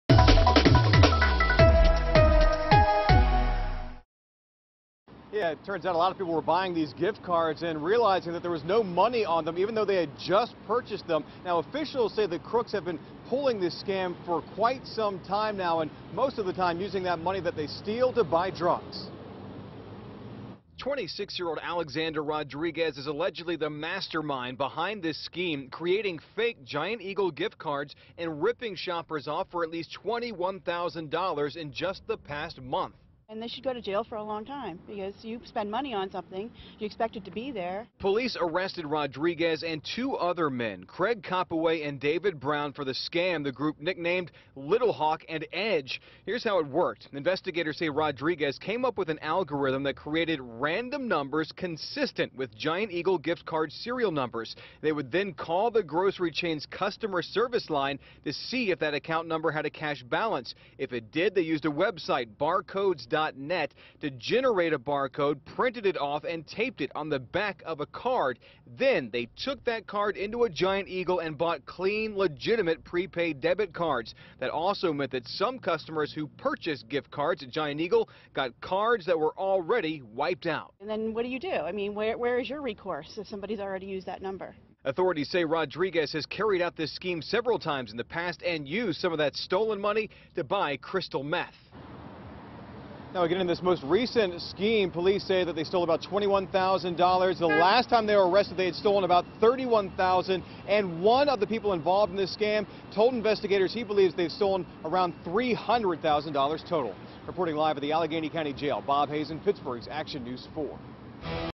Trio arrested for scheme in which thousands were stolen from Giant Eagle gift cards
Three men have been charged in connection with a scheme to rip off Giant Eagle gift card by stealing the value already purchased by customers.